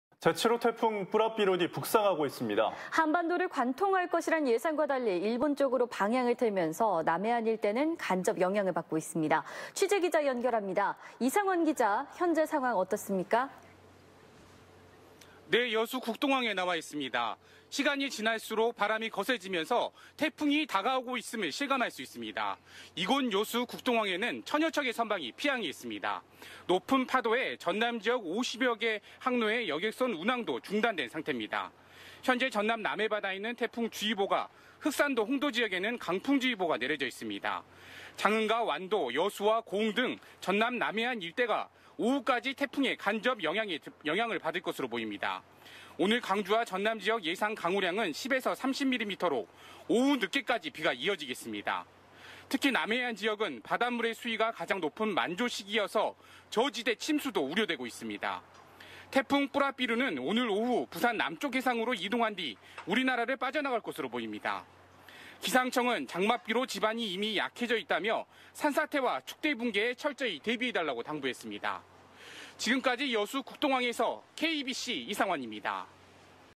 제7호 태풍 '쁘라삐룬' 북상..남해안 간접 영향
제7호 태풍 쁘라삐룬이 북상하고 있습니다.

한반도를 관통할 것이란 예상과 달리 일본 쪽으로 방향을 틀면서 남해안 일대는 간접 영향을 받고 있습니다.

취재기자 연결합니다. 이상환 기자, 현재 상황 어떻습니까.